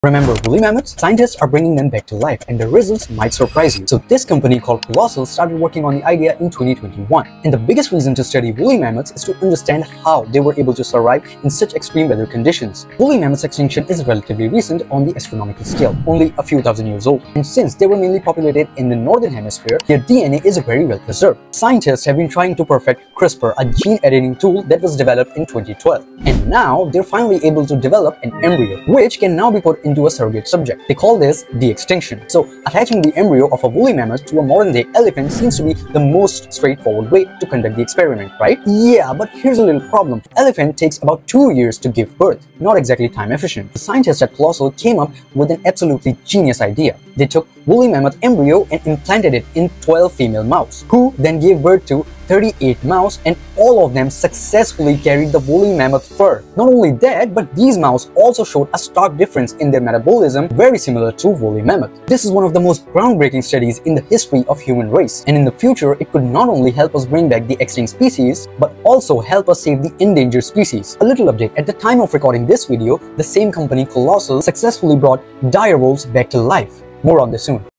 Woolly Mammoth Is Coming Back? This Will Blow Your Mind!
Scientists are bringing back Woolly Mammoths using cutting-edge gene editing! Colossal, a biotech company, is leading this de-extinction revolution. This groundbreaking development could change how we save endangered species. Also, guess what? Dire Wolves are back too. Full story in this short!

Follow for more crazy science & social stories!